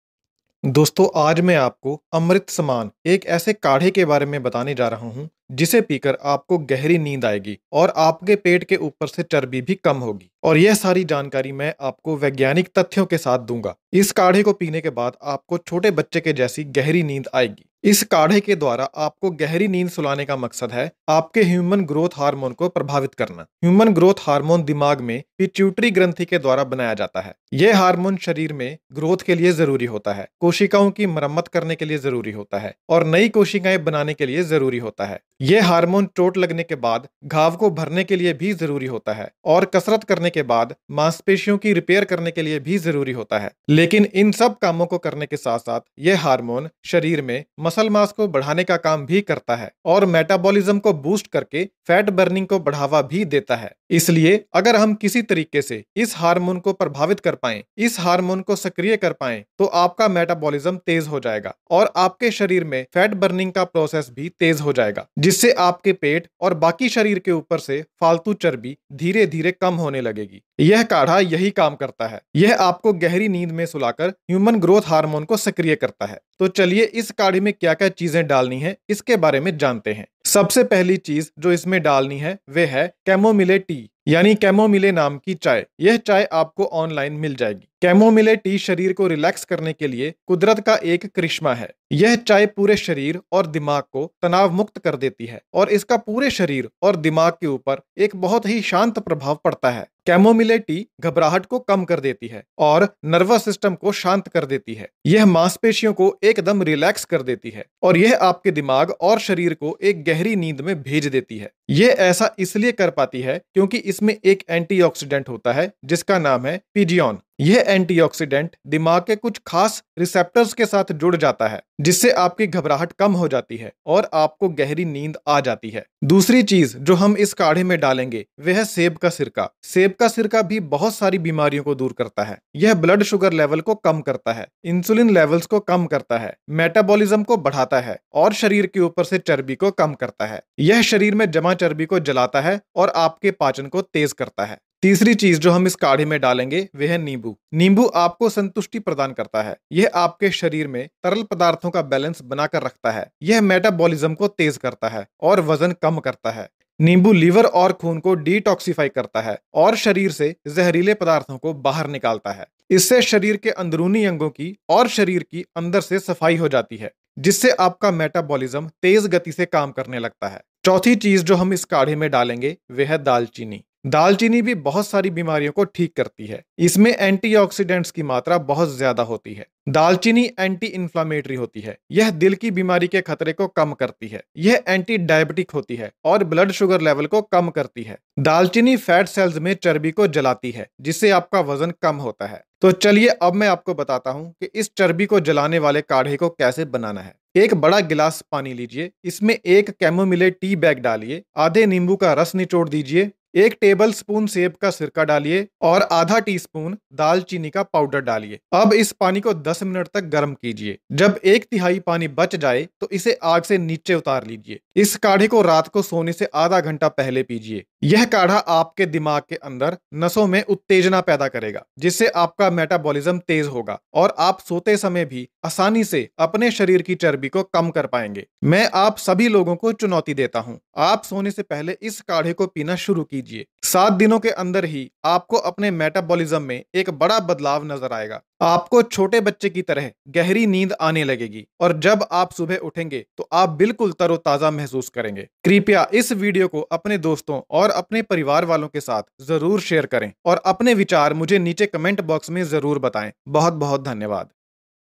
सोने से पहले एक कप... गहरी नींद पाएं और पेट की चर्बी घटाएं, For Deep Sleep And To Reduce Belly Fat
This video is about a mixture that will bring deep sleep and reduce body fat while sleeping.